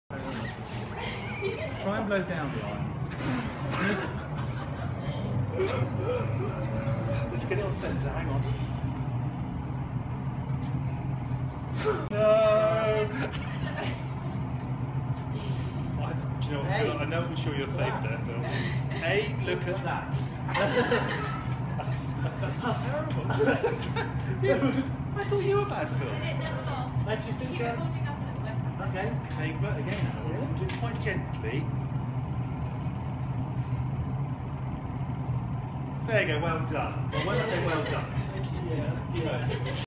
Dan blowing glass
Not sure what Dan is making here. Comments welcome...